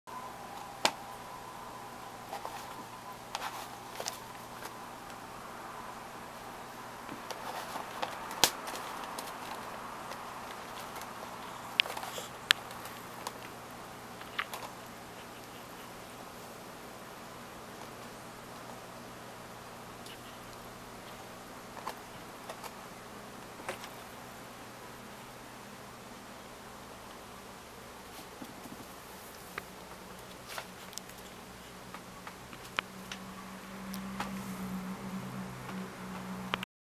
Alexandra vs Kleenex
Alexandra rips up some tissues sticking out of the box of tissues.